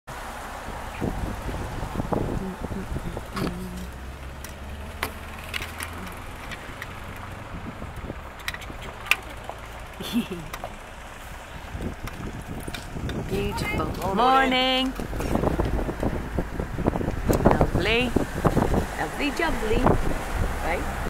English warm Horsey Hello
While visiting England East Sussex to be exact I discovered the magic of diversity as packed and crowed as it is compared to Canada, horses and their riders have a right to be. What a freedom and a lovely warm hearted Good Morning.